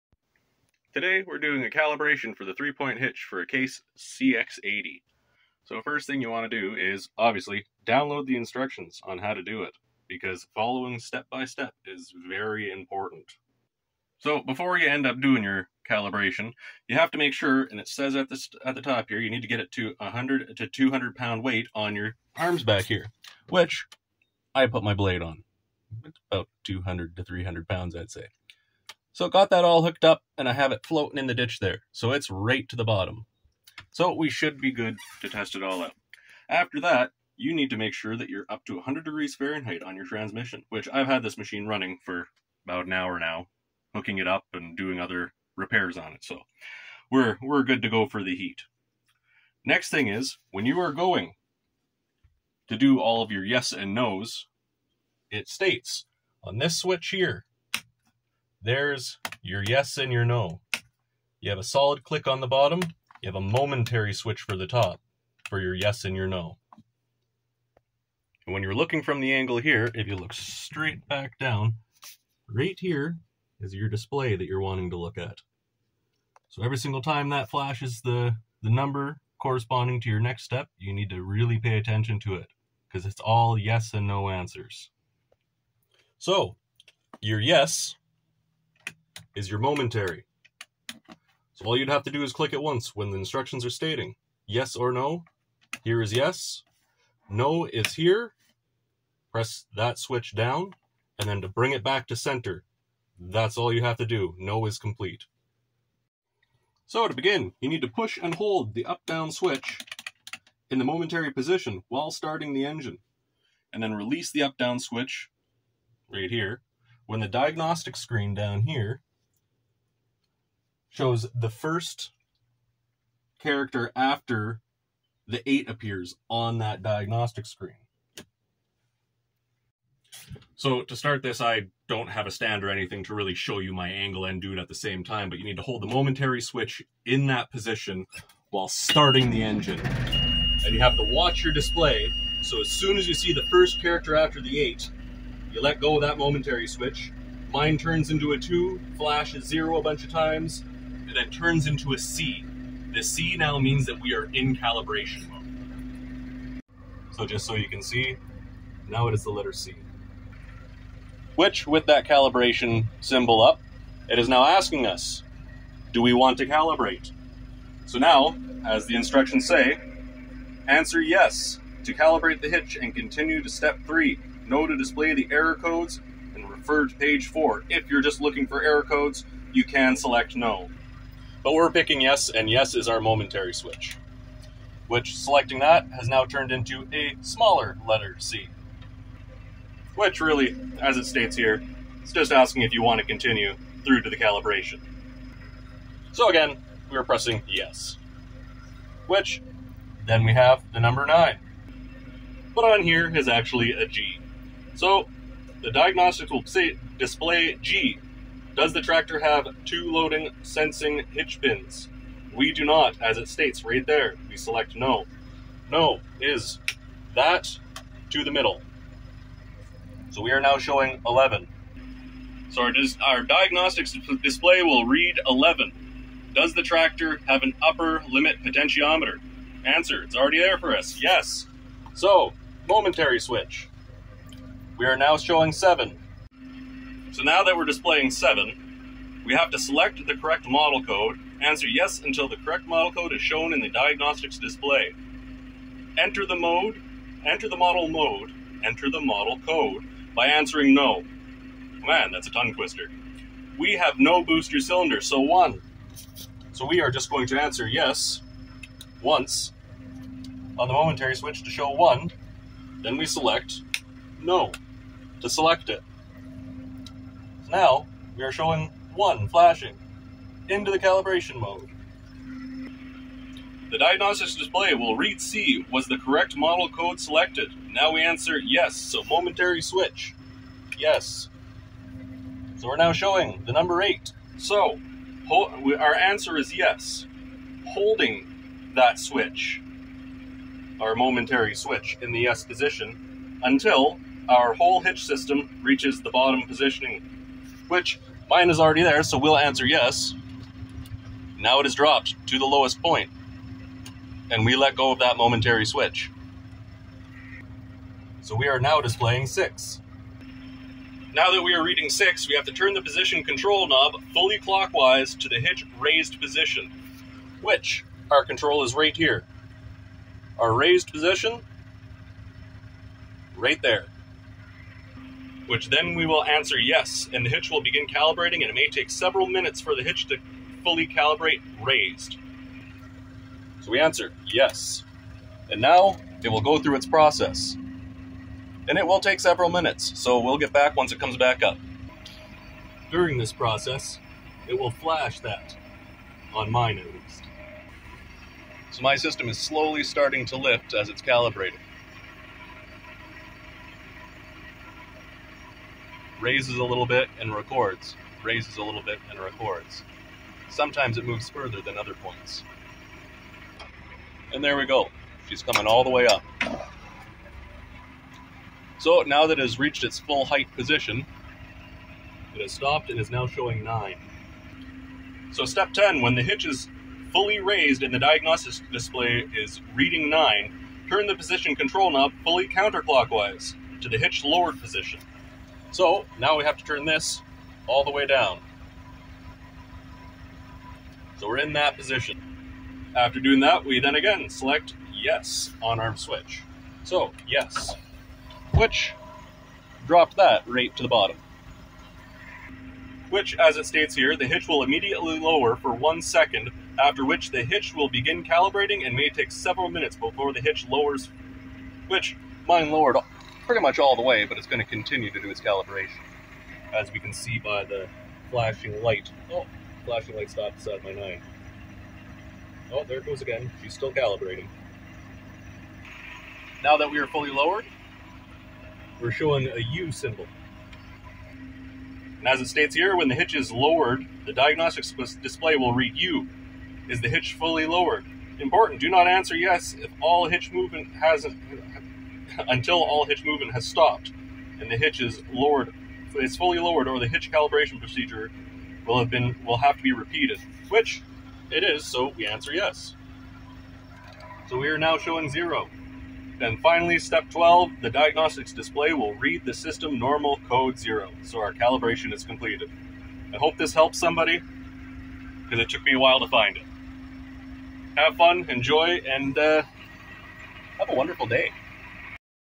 Case IH CX80 3 Point Hitch Calibration
My potentiometer decided it was its time to stop working so I figured this was a good time to recalibrate the 3-point hitch.

To my understanding this will work for all the Case IH CX series 3 point hitch systems, but make sure you find the correct instructions for your specifically to prevent issues or errors.